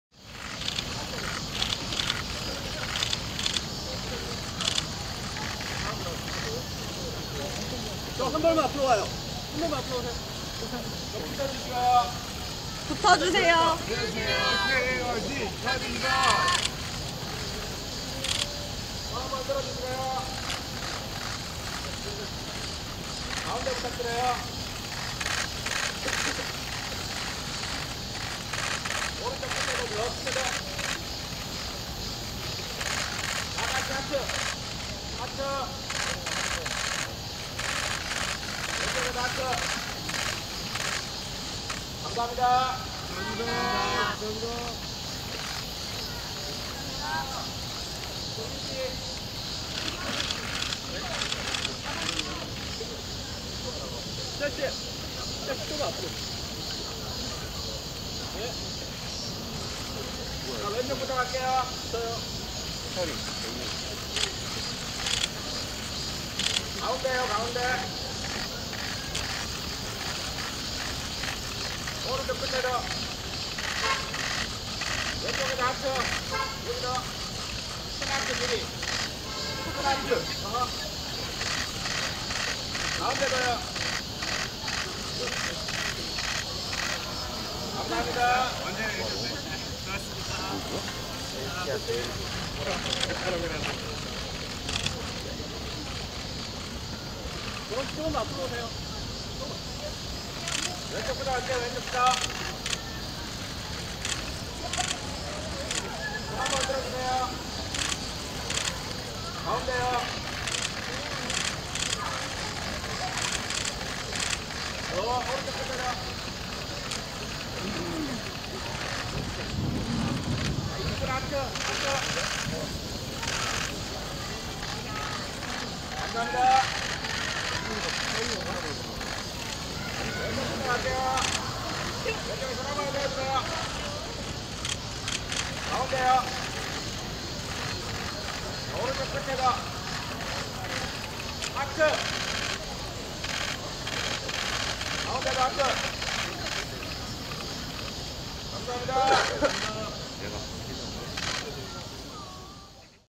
[SSTV] KARD·박보람·박재정·앤씨아, 아침에도 포텐 터지는 꽃미소 (뮤직뱅크)
‘KBS 뮤직뱅크’ 리허설이 21일 오전 서울 영등포구 KBS 신관에서 진행됐다.

이번 방송에서는 정용화, 엑소, 크나큰이 출연해 컴백 무대를 꾸민다.

걸그룹에는 KARD, 라임소다, 레드벨벳, 박보람, 앤씨아, 우주소녀, 인아 등이 출연해 무대를 펼친다.

보이그룹에는 VAV, 박재정, 보이스퍼, 스누퍼, 업텐션, 핫샷, 헤일로 등이 출연해 멋진 무대를 선보일 예정이다.

한편, 이서원, 라붐 솔빈이 진행하는 KBS ‘뮤직뱅크’는 매주 금요일 오후 5시에 방송된다.